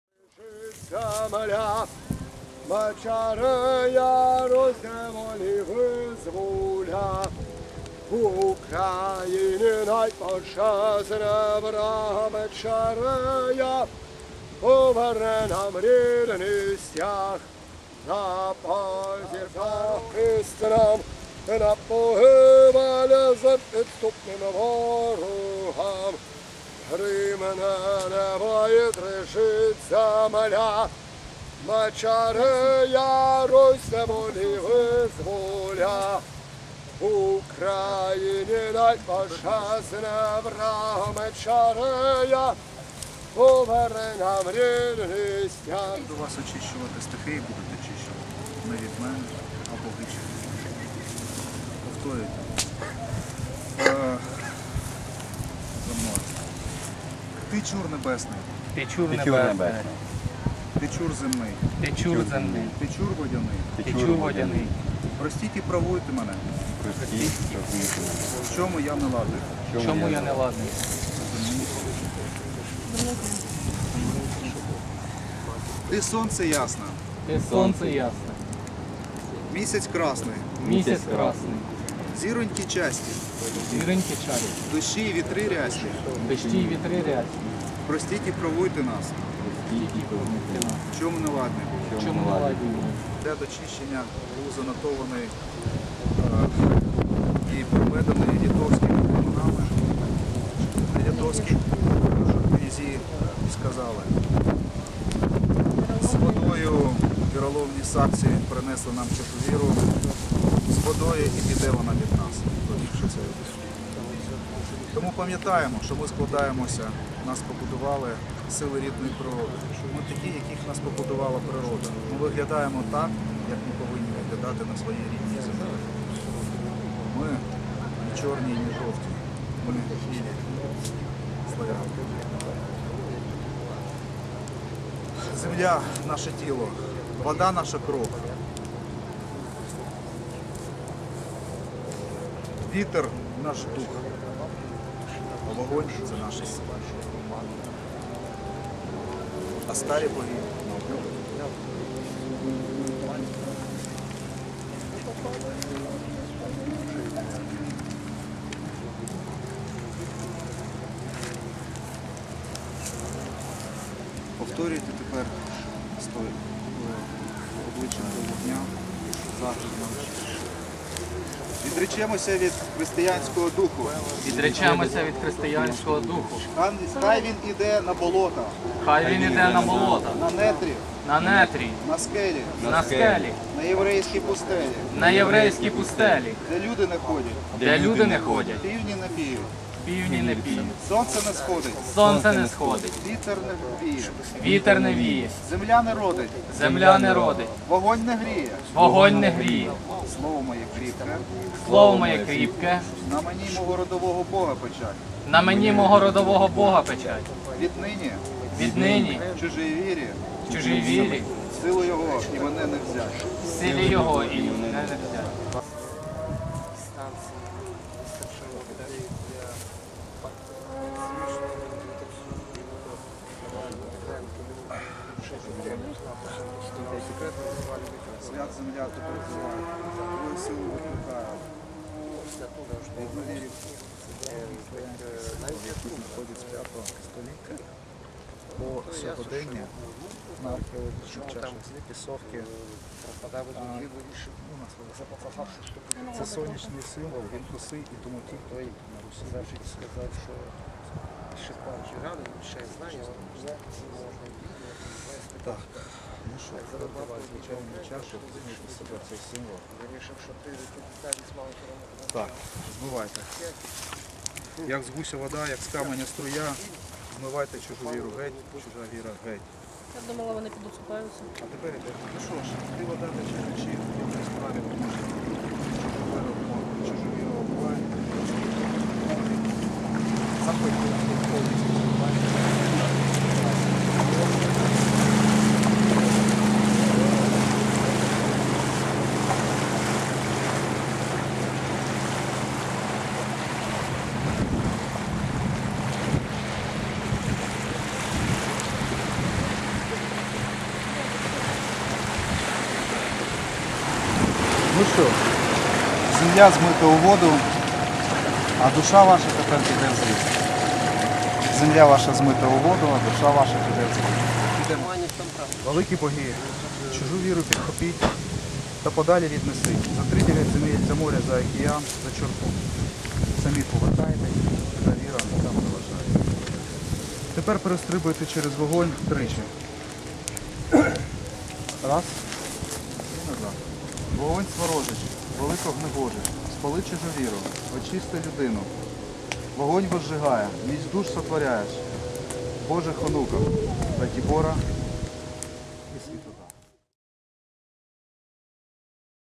Перунові дні - 4ч. відречення від +
Київська громана рідновірів-традиціоналістів КРУК відсзачила Перунові дні на дніпрових островах.